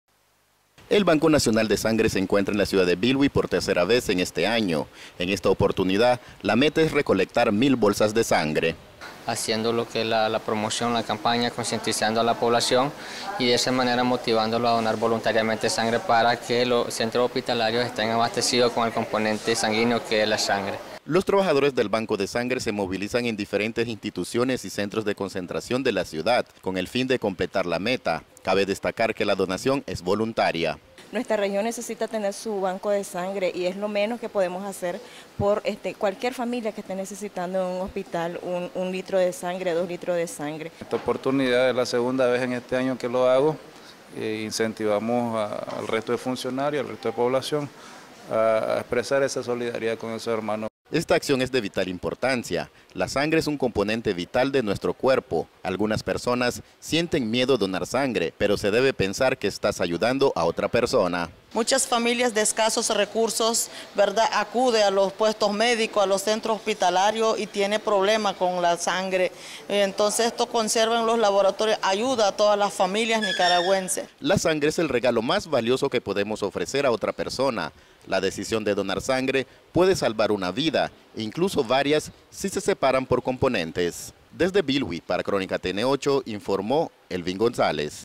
Pretenden recolectar mil bolsas de sangre en la ciudad de Bilwi, Nicaragua
Durante una semana el Banco Nacional de Sangre estará en Bilwi, la meta es recolectar mil bolsas de sangre.